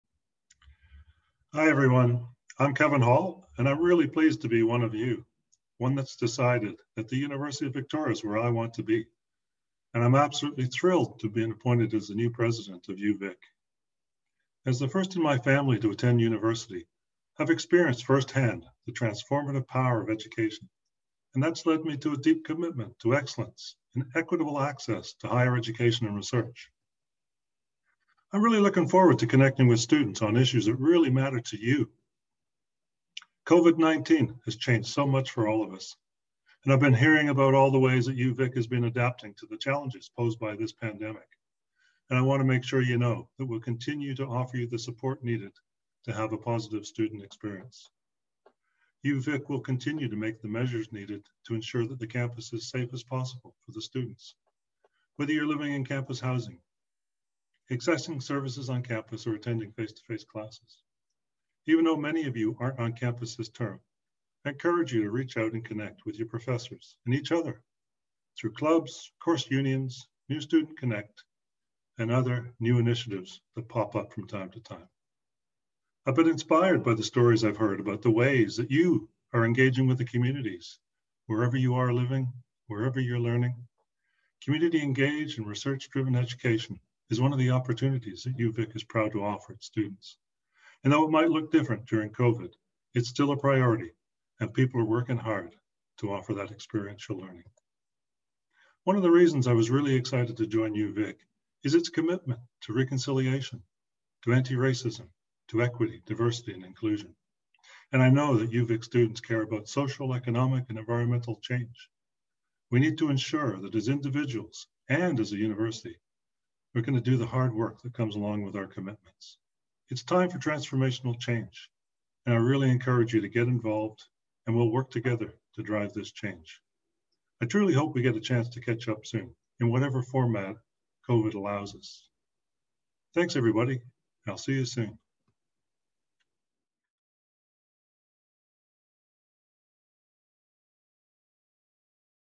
Message from UVic president Kevin Hall
"I've experienced firsthand the transformative power of education and that's led me to a deep commitment to excellence and equitable access to higher education and research. I'm really looking forward to connecting with students about issues that really matter to you."

UVic's eighth president and vice-chancellor Kevin Hall shares this message with you.